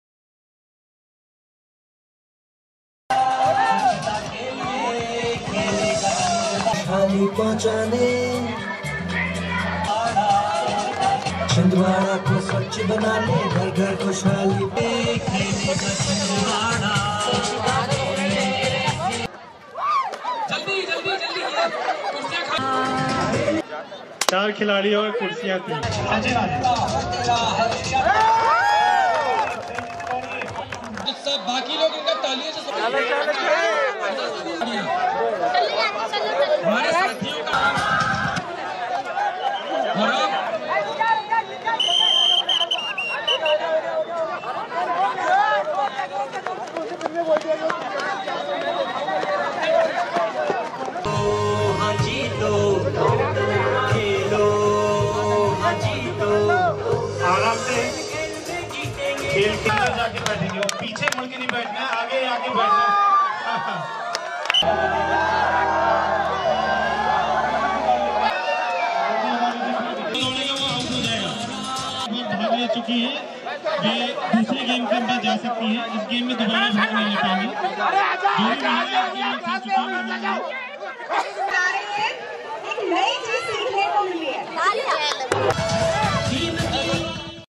Anand Utsav | Anand Sansthan | Chhindwara | Madhya Pradesh
पुलिस ग्राउंड में सफाई कर्मियों के साथ खेलो का आयोजन किया गया